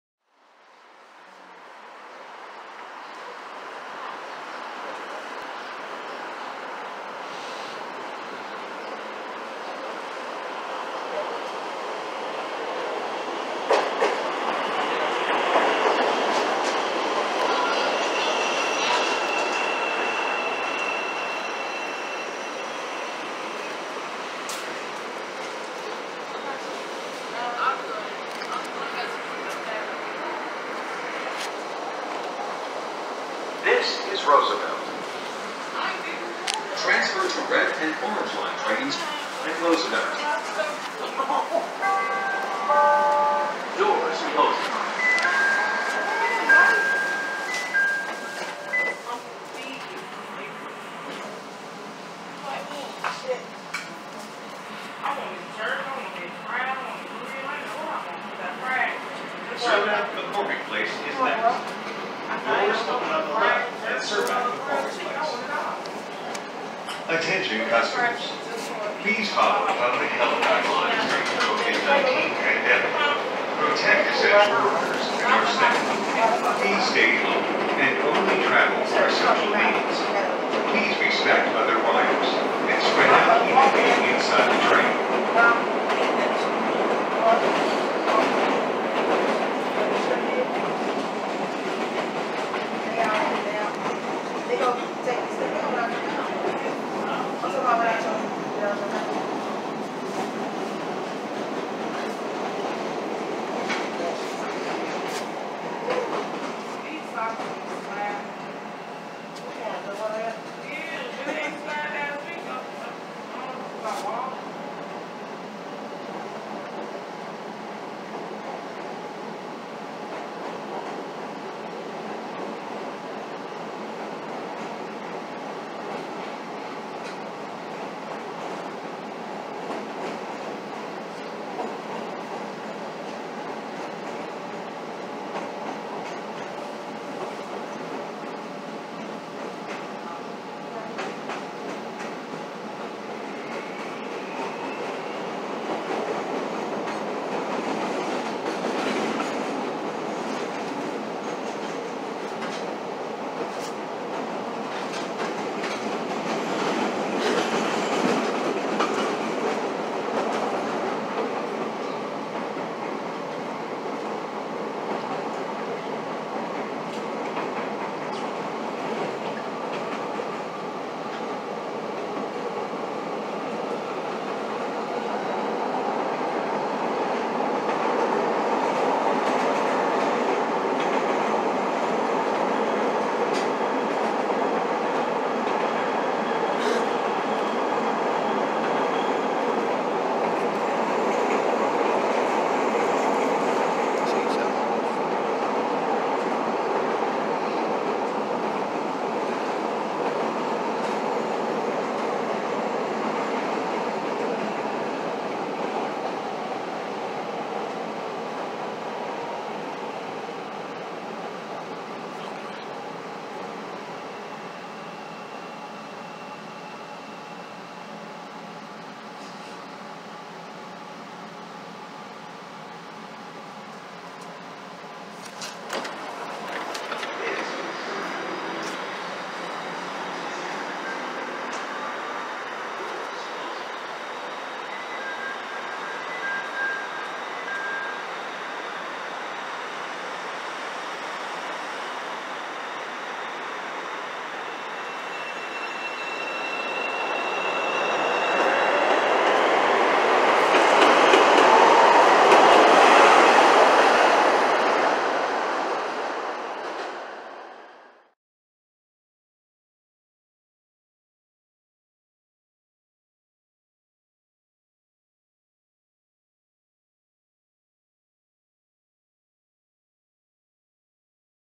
CTA Green Line, 63rd/Cottage Grove-bnd | Roosevelt to C. McCormik Place | 5000 Series | 5.19.2020
May 19, 2020 (5/19/2020) - PT. 8
- CTA 63rd/Cottage Grove-bnd Green Line - Roosevelt to C. McCormik Place - 5000 Rail Car Series Bombardier
- Recorded on: LG Aristo 3